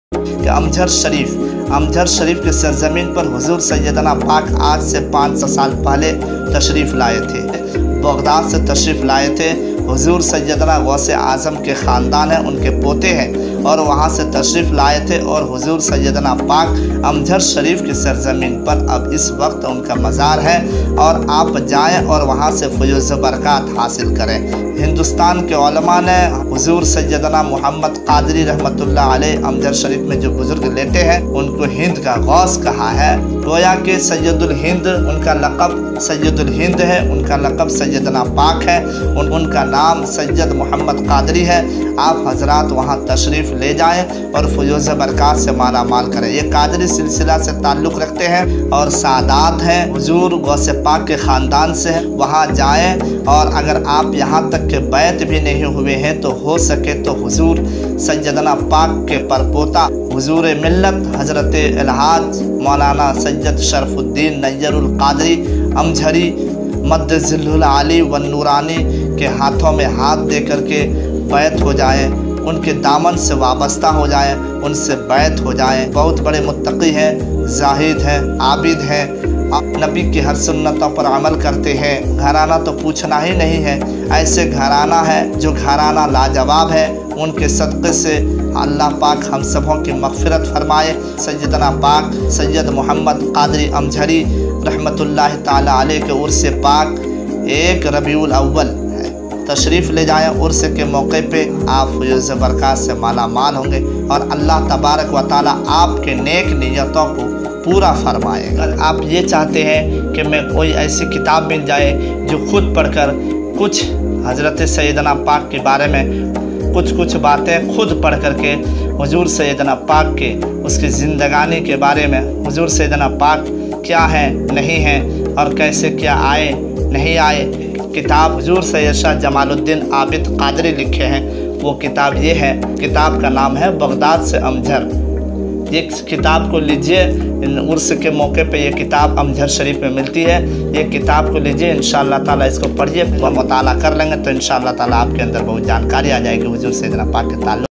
Amjhar sharif Sayedna paak Kab Aaye the? Huzur e Millat kon hain? (Mukammal video dekhiye jaaniye)
⚫Keep supporting

⚫Keep dua

⚫Keep smiling

◻️✔️Ek Qadam islam ki Aur ✔️◻️